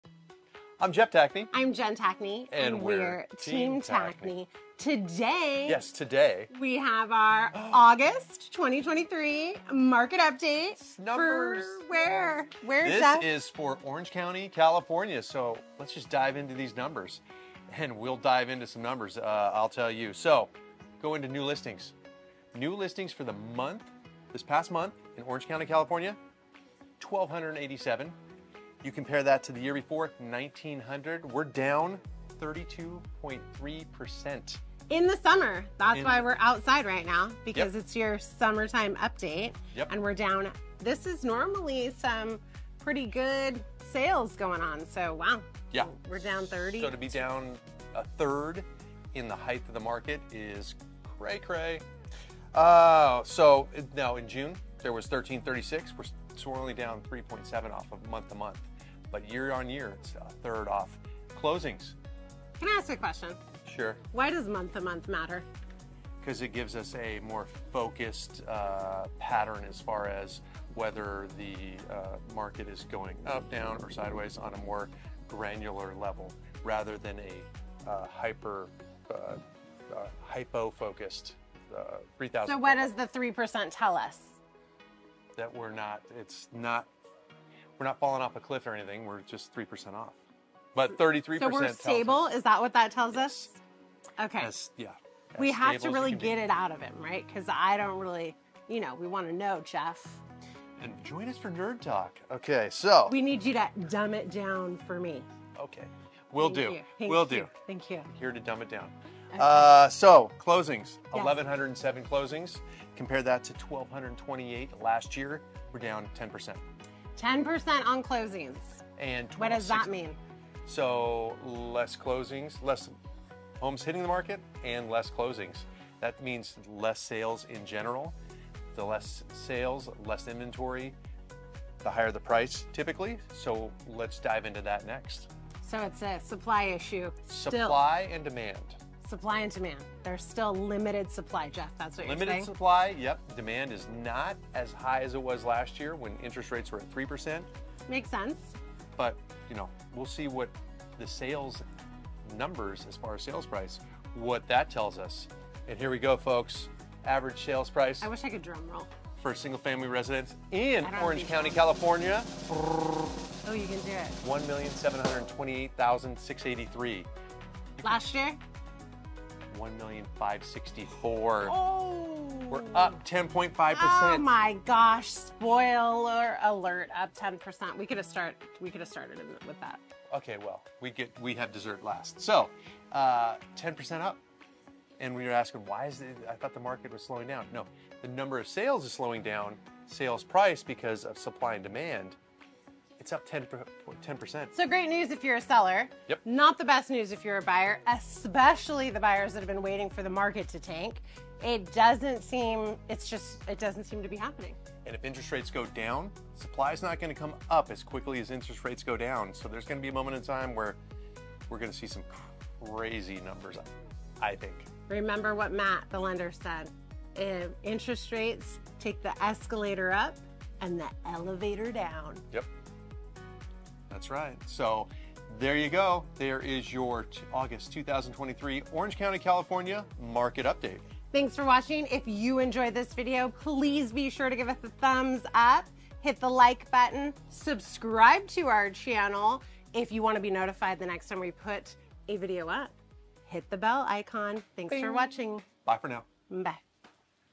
August 2023 Housing Market Update | Orange County, CA
Are you looking for the latest scoop on the Orange County real estate market? Look no further - we've got your August 2023 update right here! Join us as we take a fun and informative look into the hottest housing trends and prices in Orange County. So don't miss out on this can't-miss update!

Team Tackney - GMT Real Estate